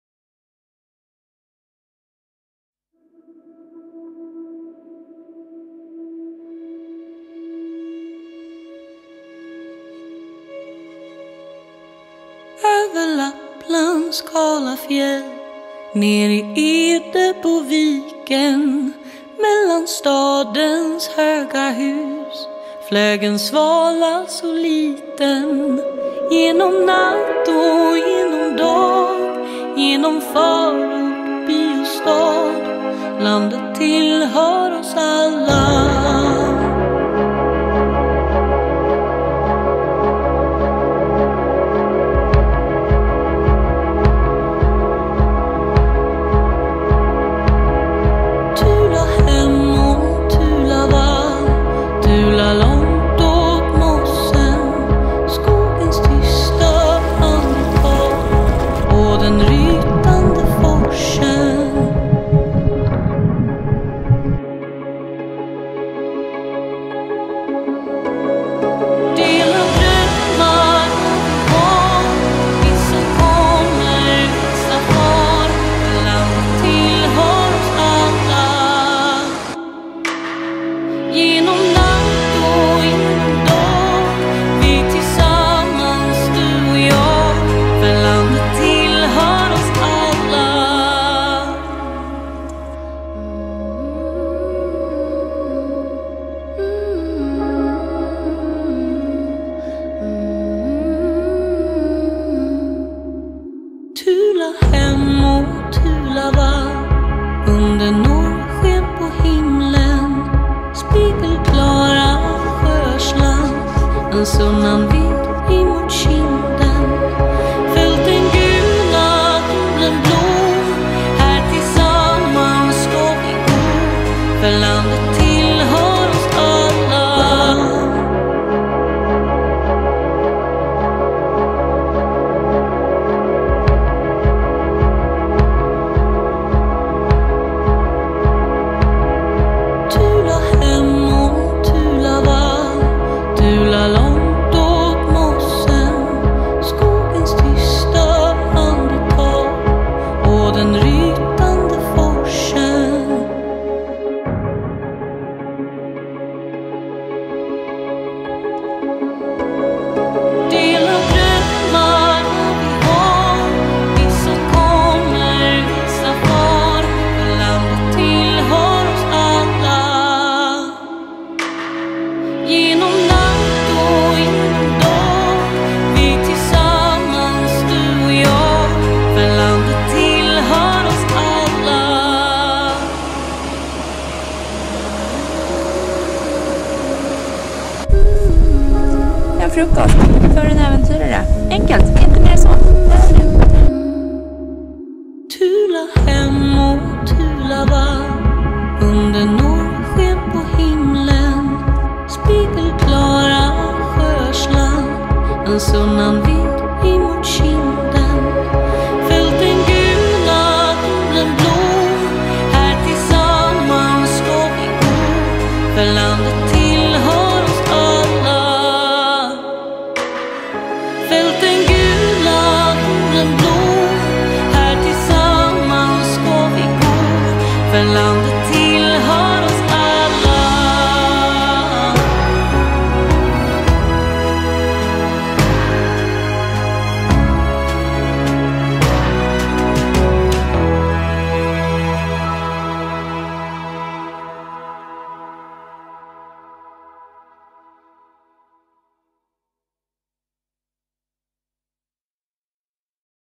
Kebnekaise 2021
Vi tre till toppen av Kebnekaise (Giebmegáisi).